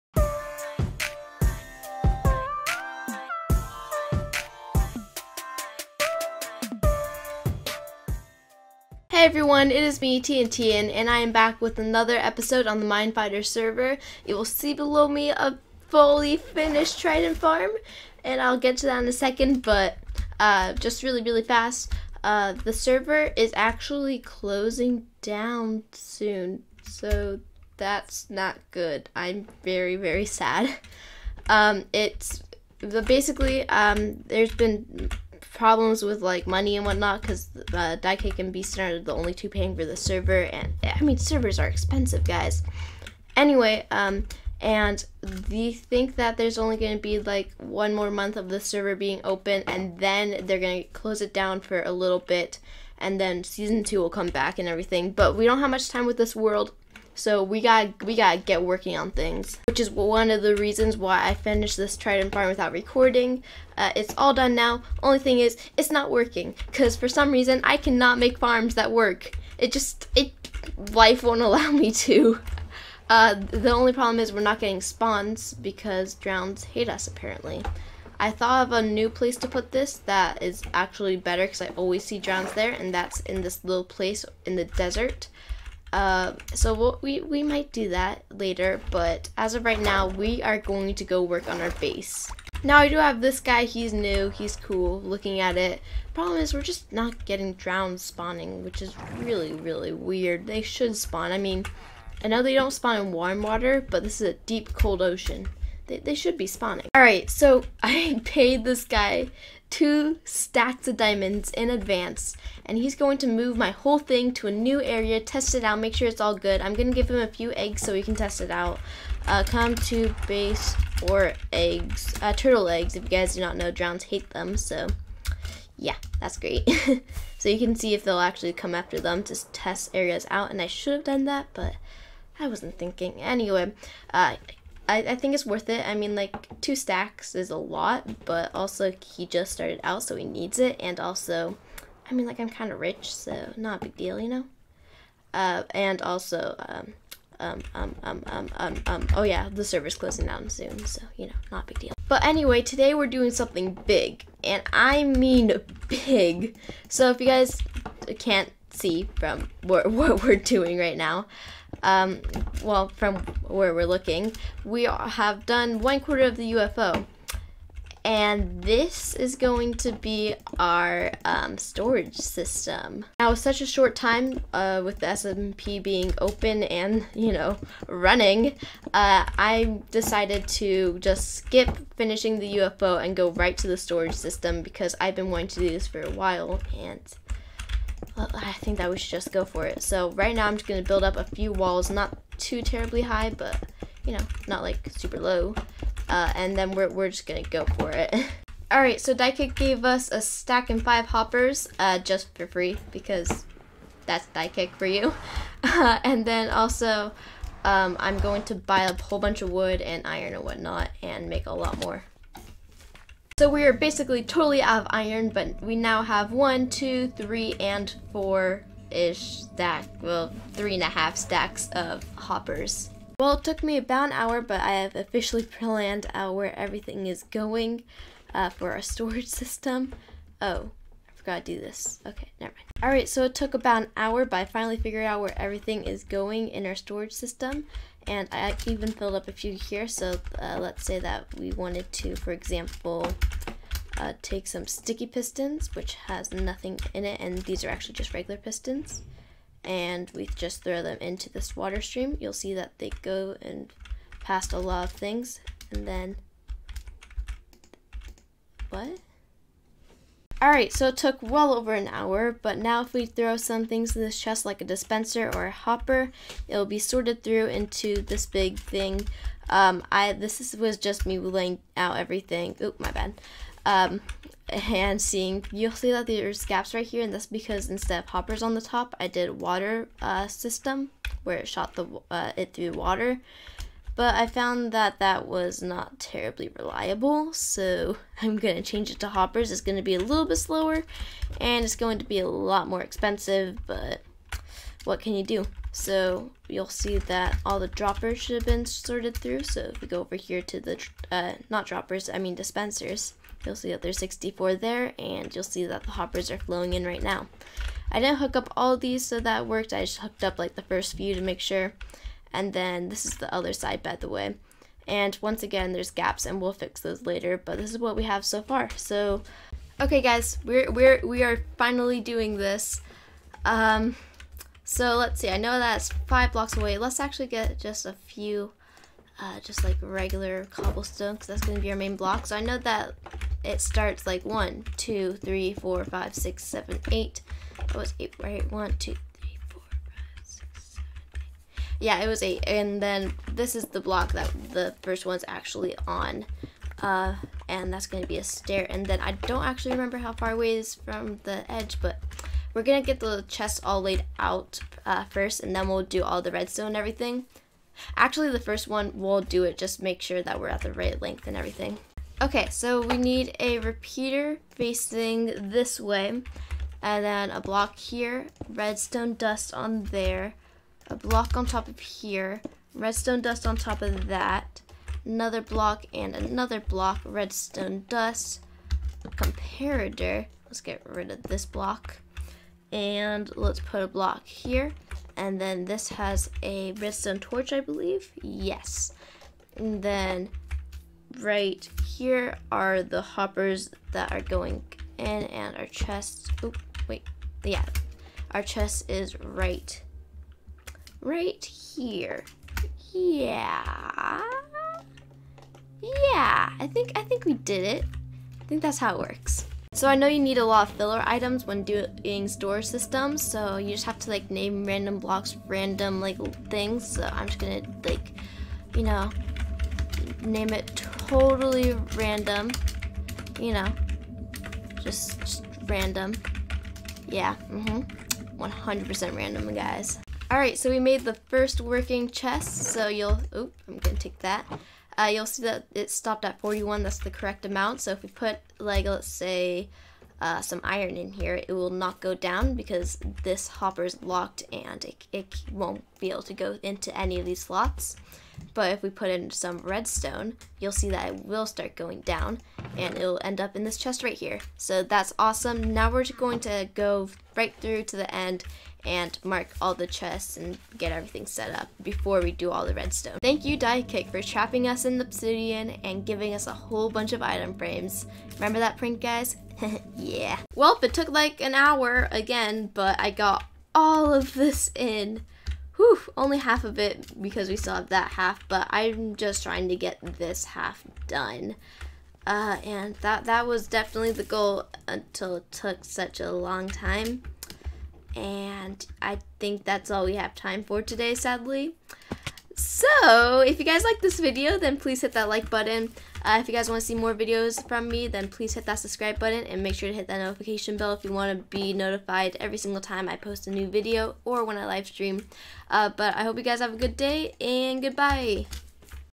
MindFighter SMP: Episode 15 - A Waist of Time...
In this video, I get started on my storage system... for no reason. Also go check out the other members on the server!!

BoxJordiHead (no channel)
JustEditing (no channel)
Quieterror (no channel)
MindFight_Mayor (no channel)

If you want to apply to join the server go to this